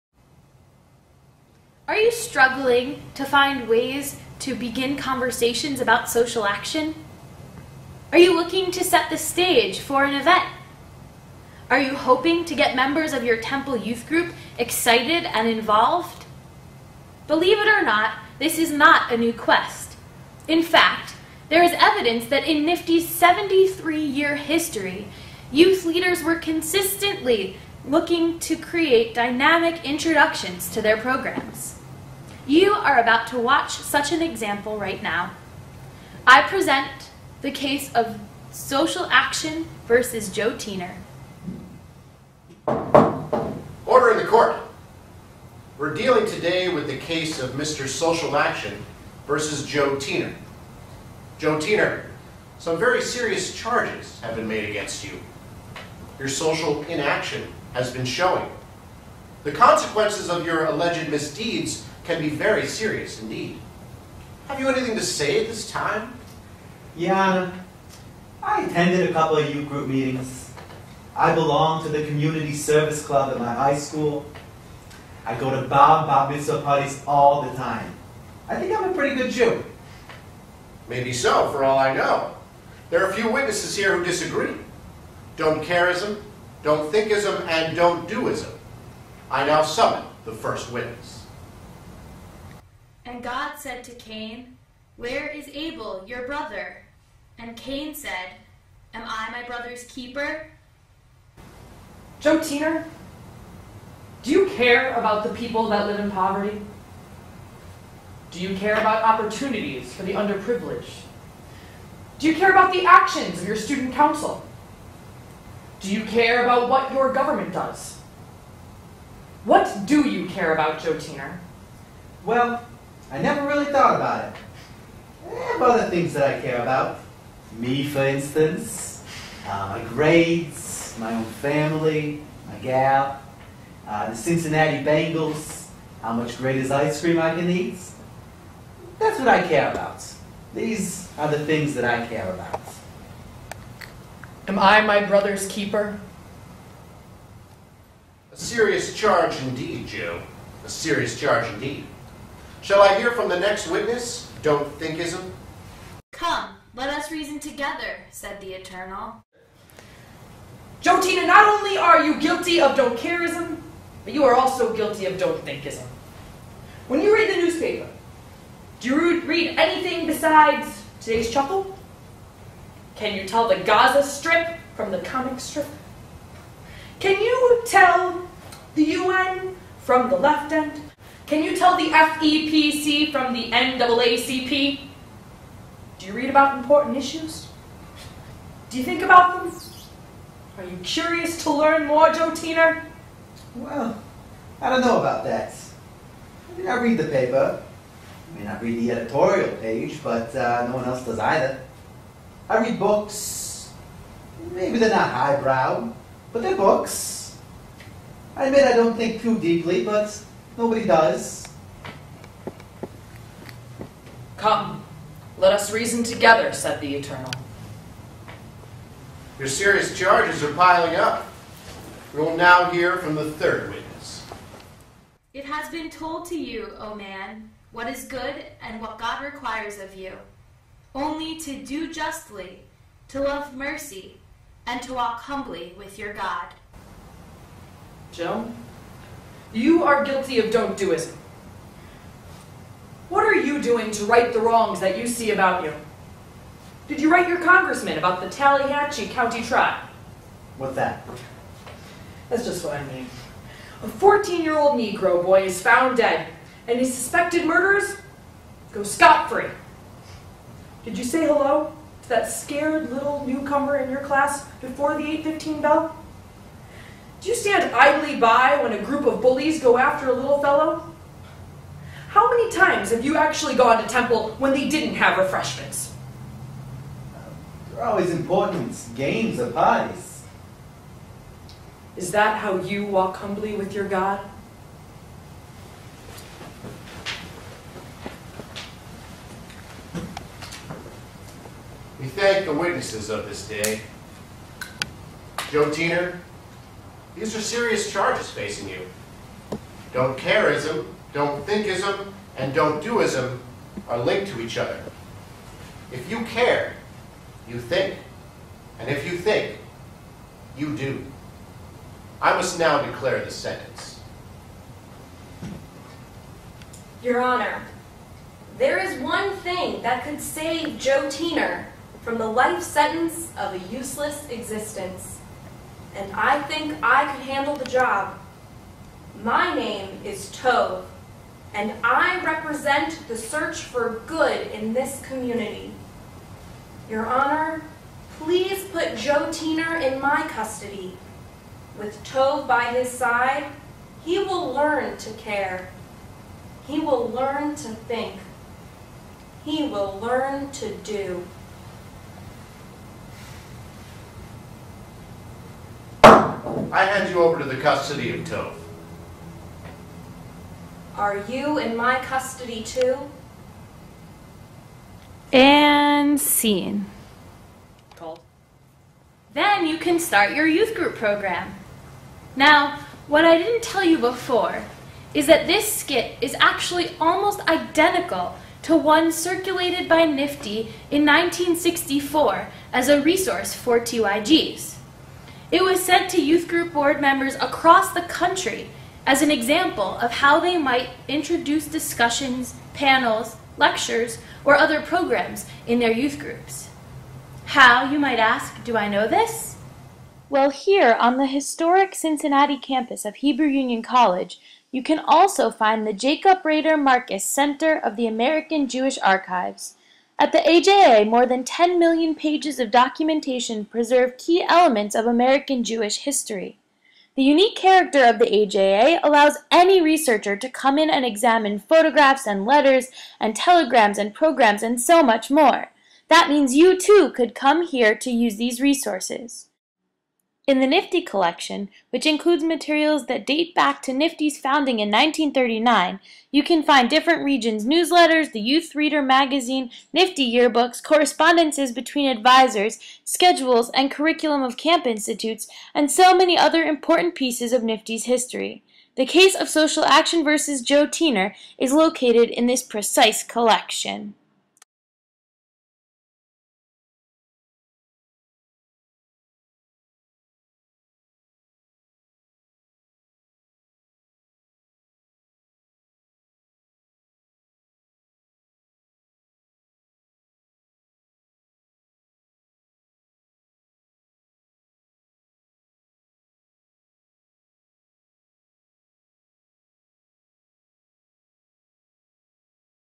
The Case of Social Action Versus Joe Teener
Documentary video created by Leah Citrin for Dr. Gary P. Zola's History 403 class at Hebrew Union College-Jewish Institute of Religion, Cincinnati, Ohio.